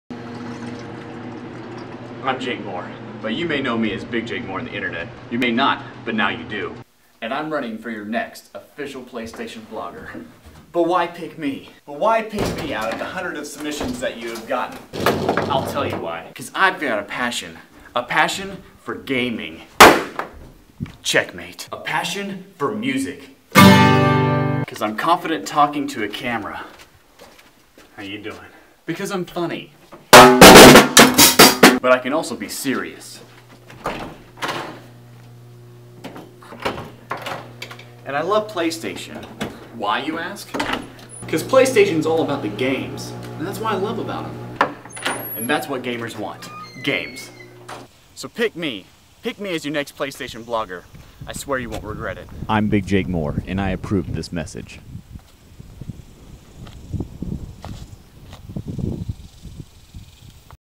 Next Playstation Blogger Contest
this is a submission video I did to be the next playstation blogger for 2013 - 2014. However i dont believe i won because i was never notified.

twitter and instagram @bigjakemoore